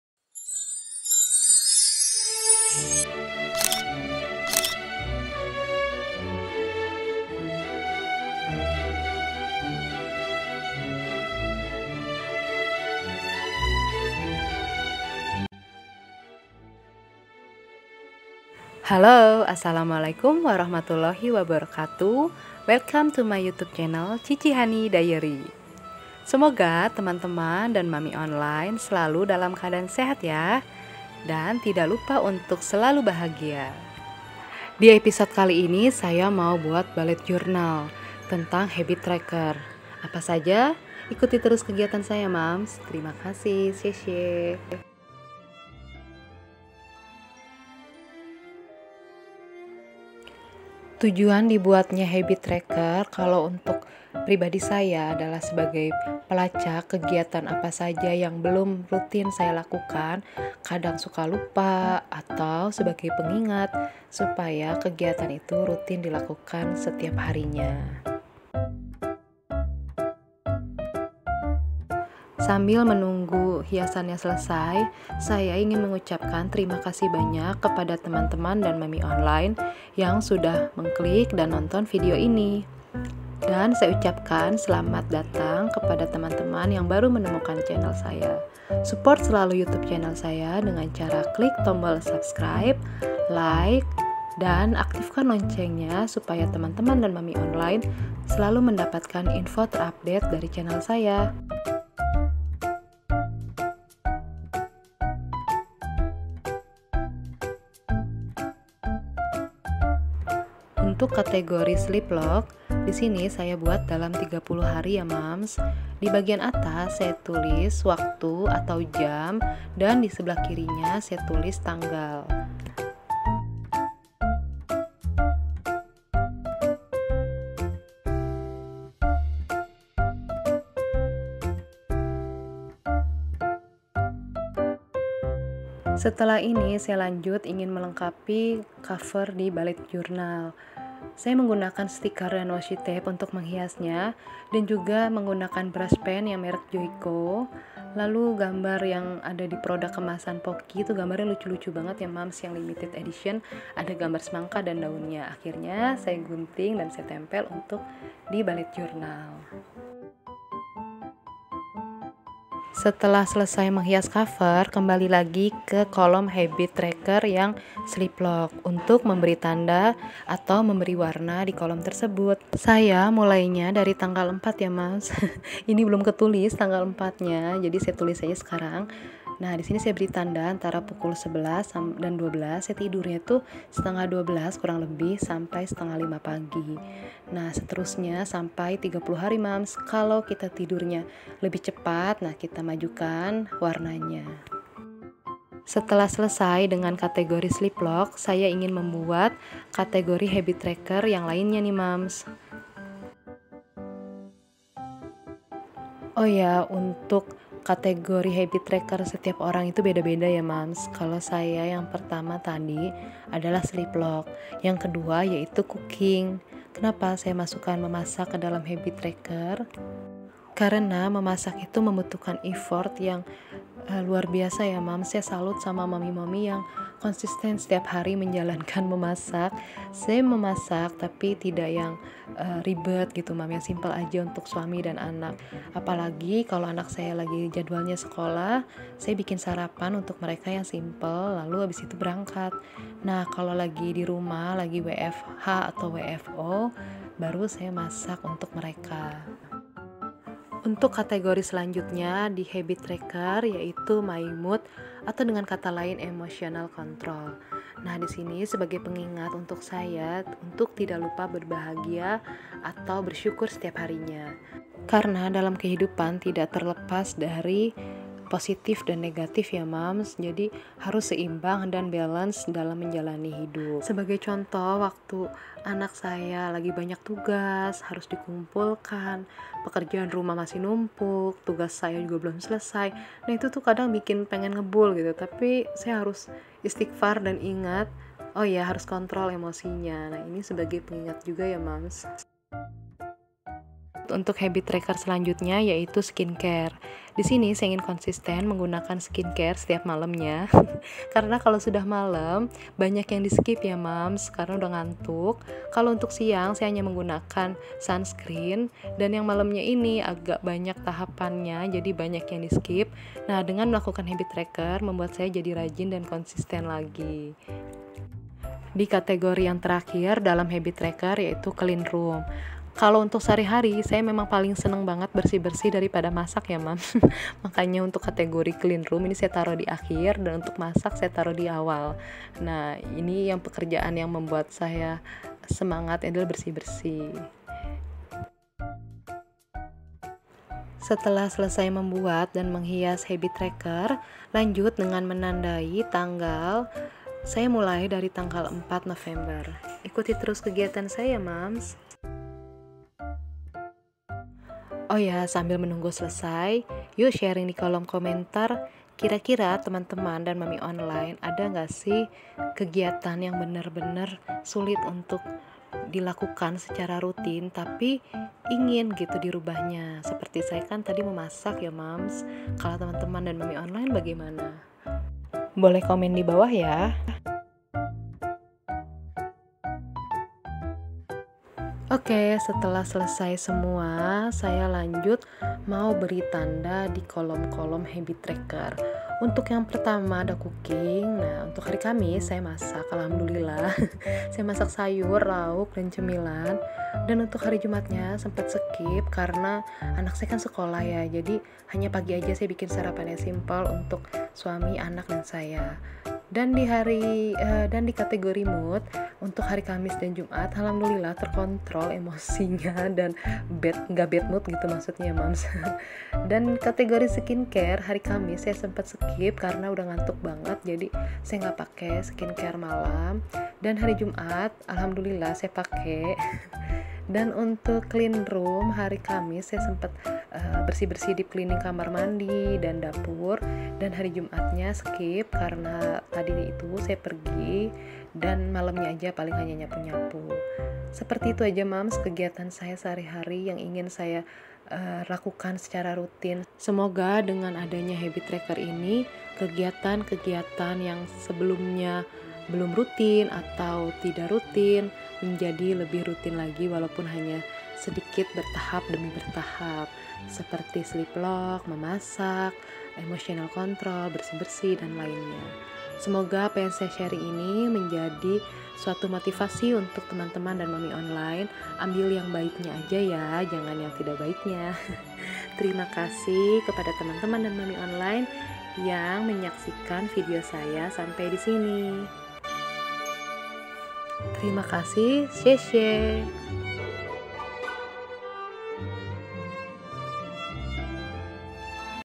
BULLET JOURNAL NOVEMBER | HABIT TRACKER CHALLENGE | JOURNAL WITH ME
Hi everyone...semoga teman-teman dan mommy online selalu dalam keadaan sehat dan berbahagia.
untuk Episode kali ini saya mau share tentang BULLET JOURNAL dengan TEMA HABIT TRACKER. semoga apa yang saya share menjadi motivasi bagi semua ya..

Terimakasih
Xie Xie

Cici Hani Diary

jika ada yang ingin ditanyakan boleh berkunjung atau berteman di ig saya

Backsong by :

BGM PRESIDENT ⤵️